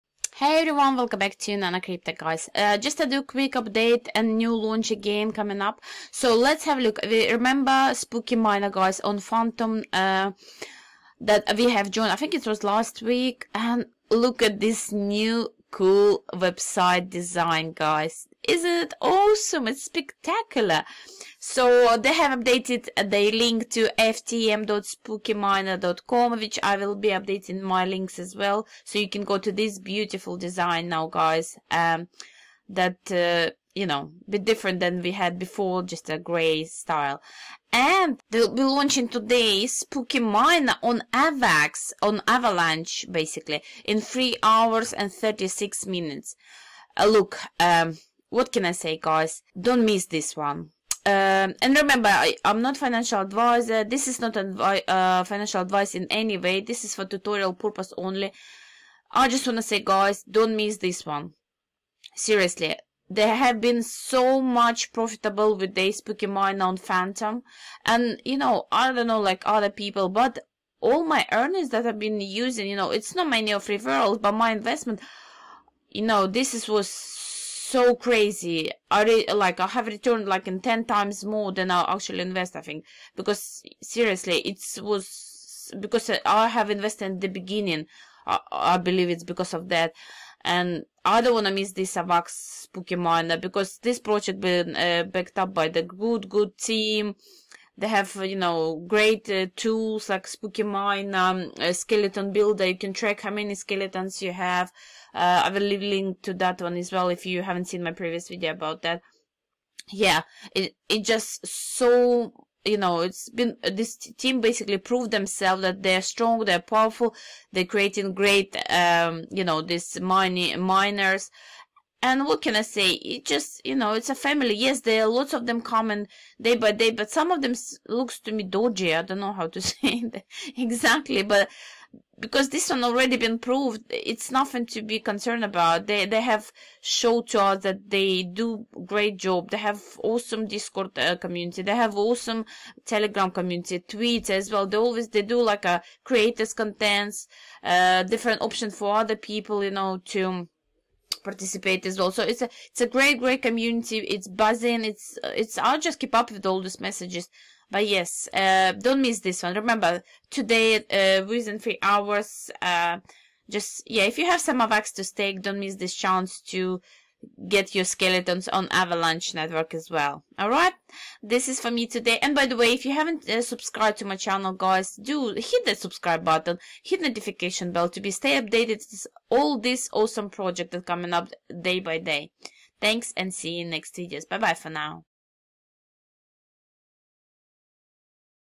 Spooky Miner On Avax Review | Build Skeletons💀 & Earn 8% AVAX Daily
In today's video, I'll introduce new project by Spooky Miner Team on Avalanche network. the have successfully launched on Fantom and I believe this would be the same, so do not MISS IT!!!

Buy Crypto and Use my referral to get a $25 USD bonus
Earn Mining:
After you begin to let skeletons dig for bones you will be able to receive up to 8% of your initial
investment every day.
Spooky Miner Compound:
Re-invest your daily rewards to build more skeletons which will non-stop dig bones out of the
ground and increase your earnings exponentially.
FEES: 6% fee on invest and sale of bones (4.5% Owners + 1.5% Marketing Wallet)
There is no fee taken when you re-invest your rewards.
Strategy :
A group of skeleton scientists developed their most profitable strategies without ever exhausting the graveyard. Yet, each having their own area to dig, they work together as a team.
Build skeletons 6 times per week and sell bones 1 time per week.
Taking profits too often will exhaust your graveyard and therefore decrease your return on investment in the short and in the long run.